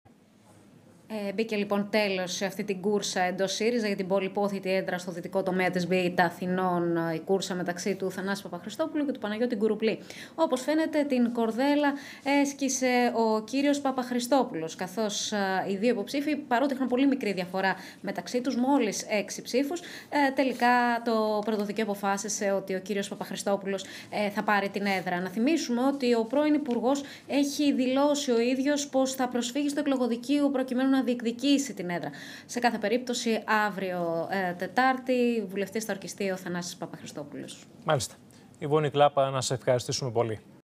Εκτός Βουλής ο Κουρουμπλής
Τέλος φαίνεται να μπήκε στο θρίλερ με την διεκδίκηση της έδρας του ΣΥΡΙΖΑ από τον Παναγιώτη Κουρουμπλή και τον Θανάση Παπαχριστόπουλο στον δυτικό τομέα της Β Αθηνών του ΣΥΡΙΖΑ, με τη θέση για το κοινοβούλιο να πηγαίνει τελικά στον Θανάση Παπαχριστόπουλο. Οι δυο υποψήφιοι βουλευτές είχαν πολύ μικρή διαφορά στις ψήφους (μόλις έξι σταυρούς) αλλά τελικά το Πρωτοδικείο αποφάσισε ότι η έδρα θα πάει στον Θανάση Παπαχριστόπουλο.